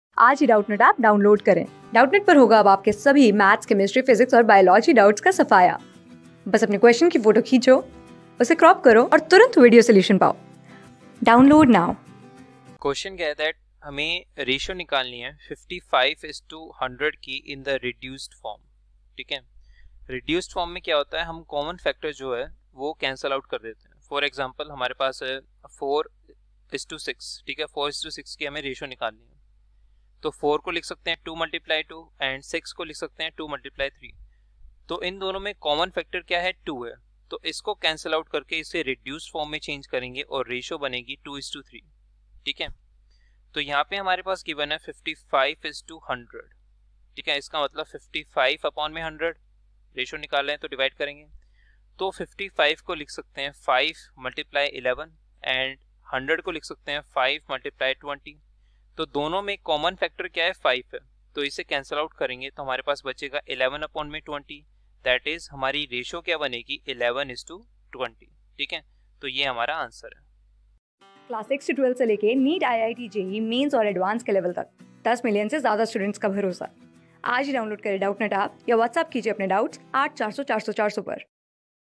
Express the following percentage as ratio in the reduced form . 55:100 | CLASS 10 | QUESTION FRO...
Express the following percentage as ratio in the reduced form . 55:100
Class: 10
Subject: MATHS
Chapter: QUESTION FROM STD. IX
Board:IIT JEE

👉 Have Any Query? Ask Us.